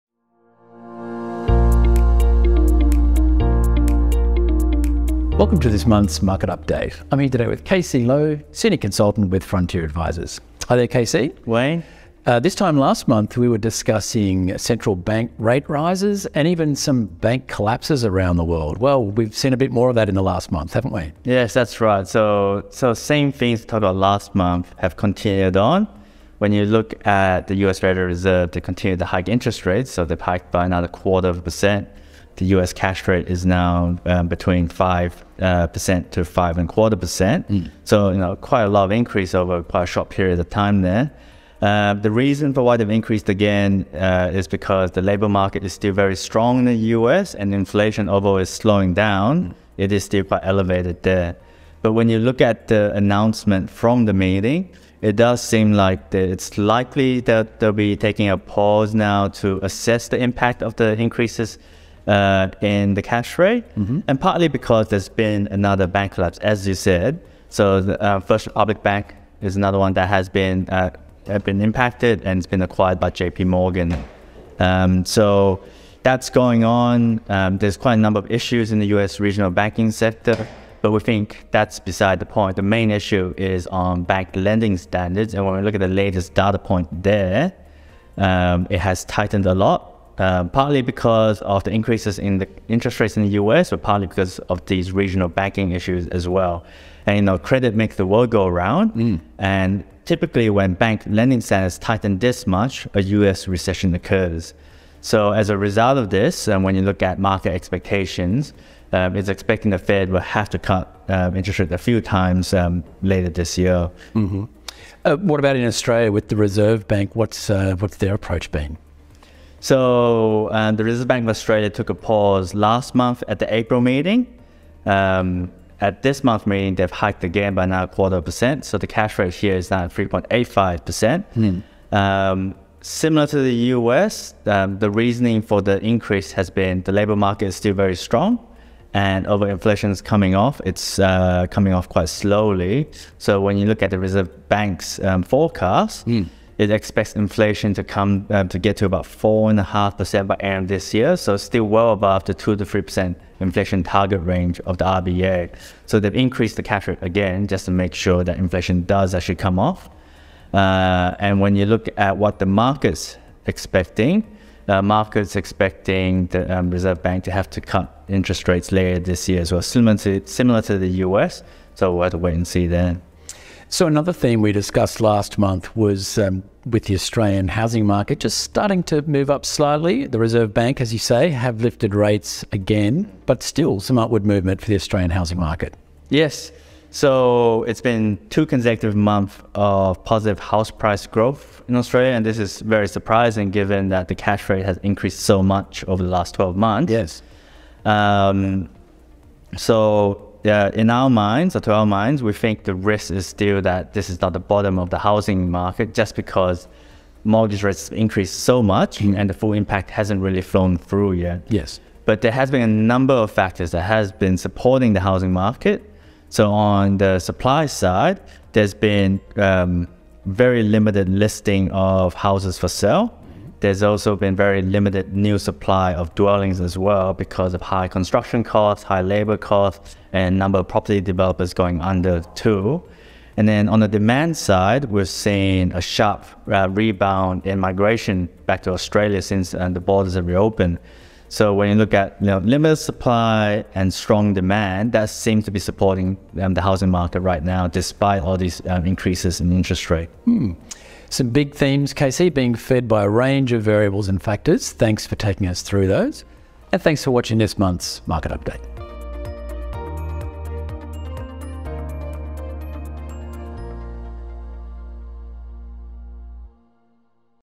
Frontier Market Update | May 2023
In this May market update, Wayne Sullivan and KCC Low say there have been further rate hikes by central banks and more bank collapses. In the US, labour markets are still strong and inflation is still elevated but slowing, and bank lending conditions have tightened materially which is expected to lead to the economy slowing down.

They discuss the prospect of the US Federal Reserve Board cutting rates later this year. In Australia interest rates continue to be increased by the RBA for similar reasons. They explain why the Australian housing market is still being supported by a range of factors. Lastly, they state their belief we are not at the bottom of the market, given the impact of higher mortgage rates has not yet flowed through.

Frontier Advisors provides asset consultancy services to the First Super Trustee. The Trustee also engages Frontier Advisors to provide market and investments updates to members of First Super.

Frontier Advisors is responsible for the information in this presentation.